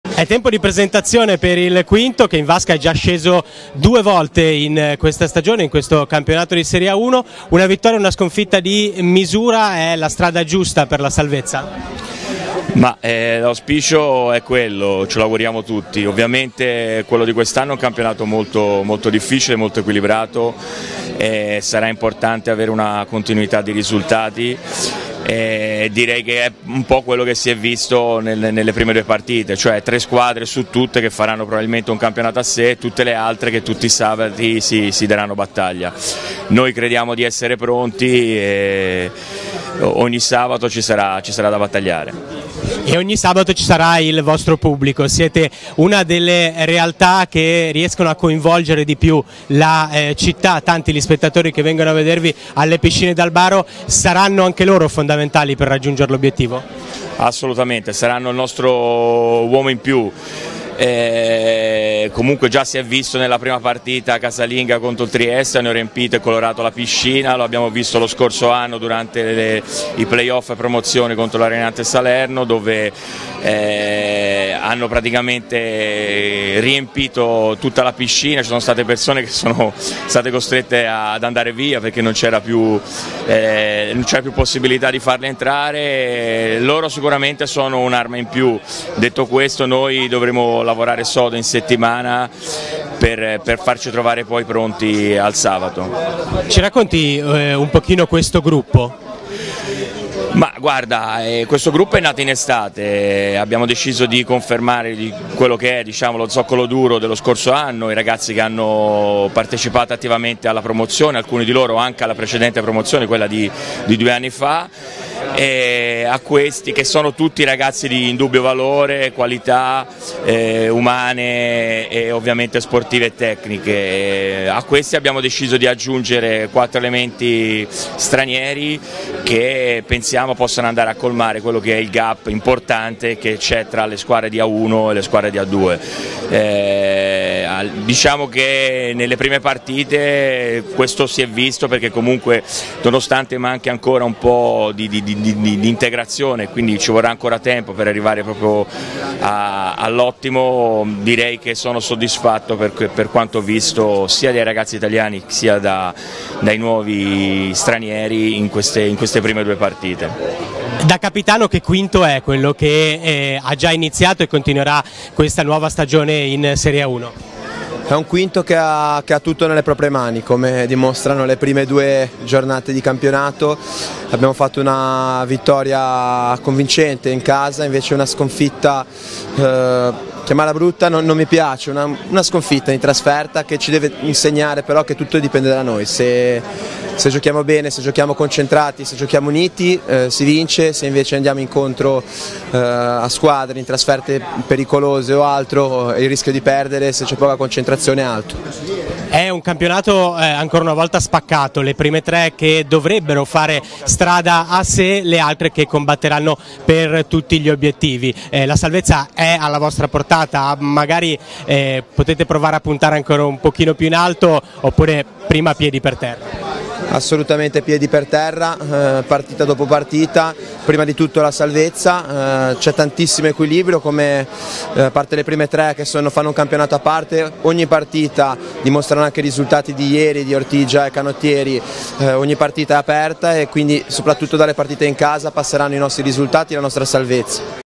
Obiettivo salvezza per lo Sporting Club Quinto in A1
Presentata la nuova Sporting Club Quinto che vuole la salvezza per mantenere Genova in A1 dopo la promozione della scorsa stagione. Ai nostri microfoni coach Gabriele Luccianti e il capitano Luca Bittarello.